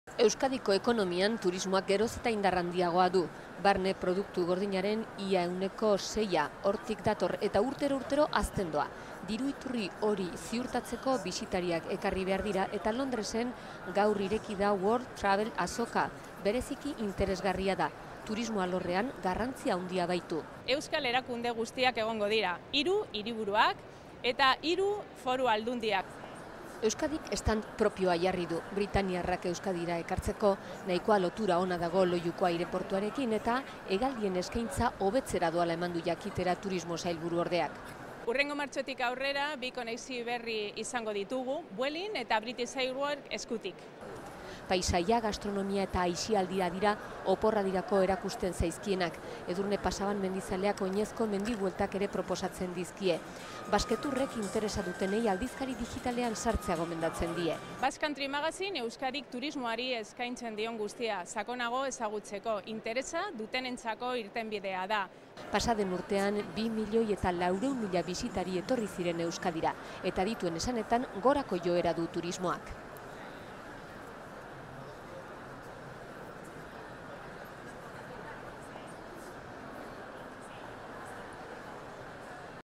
Euskadi Londreseko World travel marketen , turismoaren zita nagusia
Munduko Turismo azoka nagusienetakoa da Londreskoa. Estreinekoz bere stand propioa jarri du Euskadik. Basque Country leloarekin erkidegoko 3 lurraldeak euren eskaitza eraman die britaniarrei.